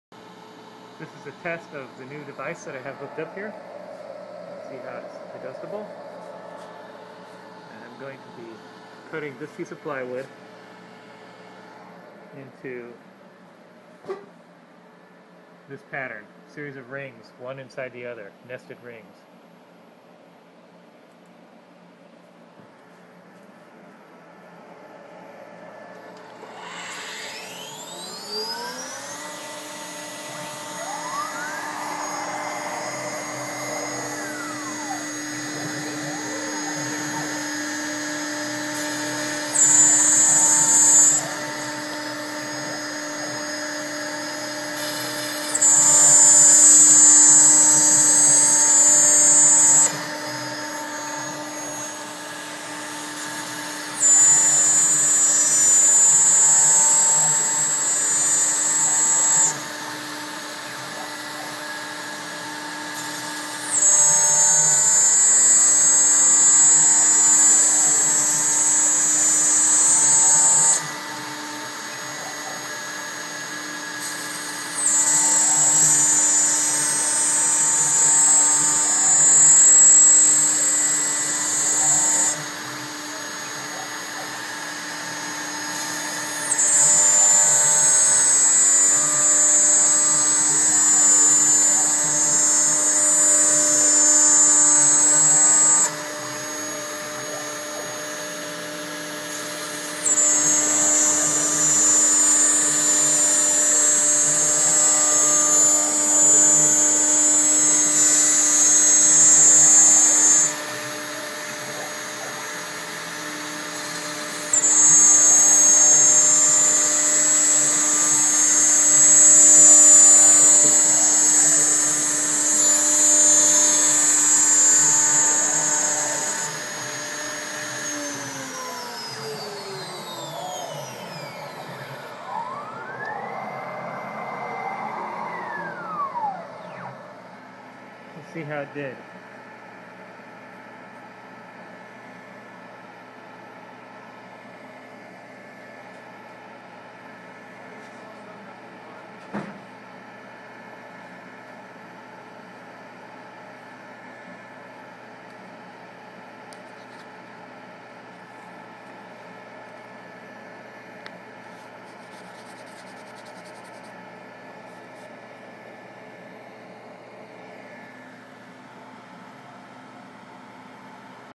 Testing my WidgetWorksUnlimited CNC Pressure Foot
WidgetWorksUnlimited makes a very handy attachment for a CNC router that enables the user to make small parts without the usual dangers and quality issues. As seen in this test, the CNC Pressure Foot Clamping Attachment worked well.

My friend James Yonts and I made the mounting bracket for my CAMaster.  WidgetWorks only makes mounting brackets for various ShopBot models, so if you own something else, you'll have to come up with your own mounting design.